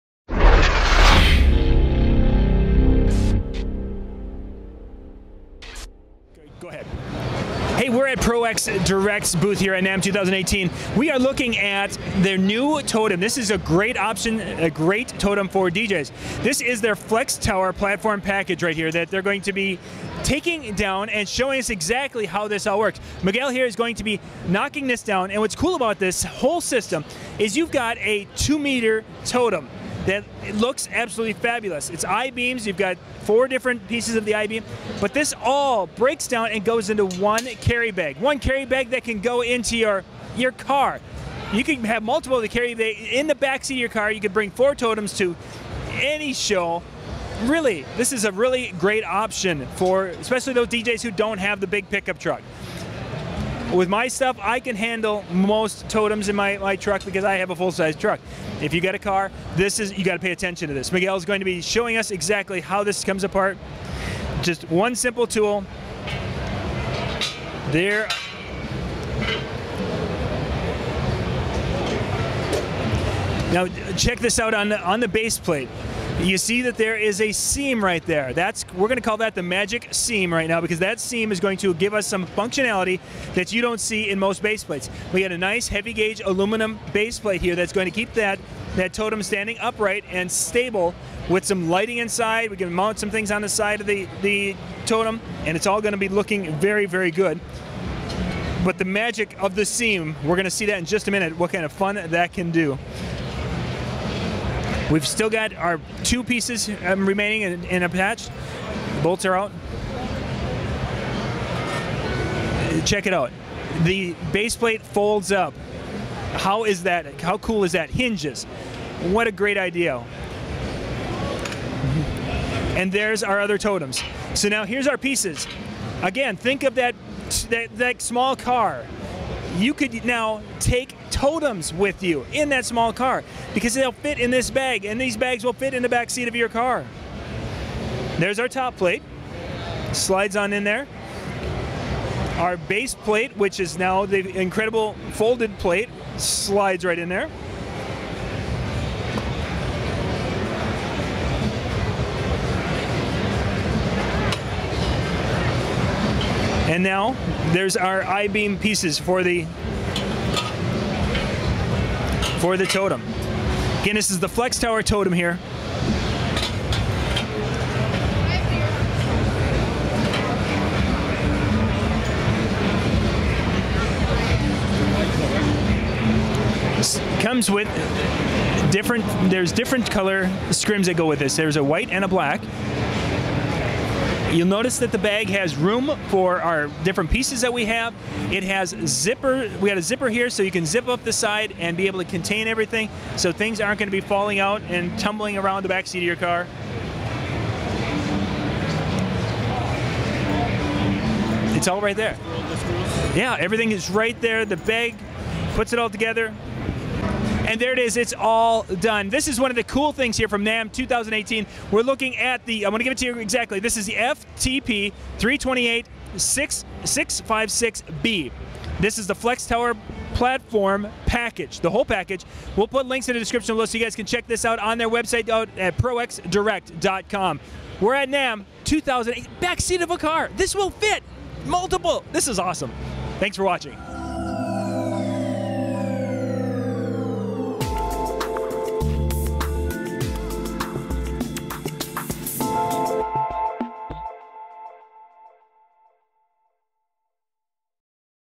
ProXDirect Flex Tower Platform Totem Package Demonstration DJ Lighting Stand | Disc Jockey News
ProX XT-FTP328-656-B FLEX TOWER PLATFORM TOTEM PACKAGE
Make your DJ display rise above the rest with the ProX Direct B Flex Tower Platform Totem package. The innovative flex tower is a complete package to create a single mount for disc jockey lights, speakers or various other display uses. This package comes with everything you need to make a single 2 meter (6.56 ft) platform or two 1 meter (3.2 ft) towers.  Tidy up the look with the black scrim sleeve or up-light the white scrim sleeve for a custom color effect. Perfect for any disc jockey, stage or display need.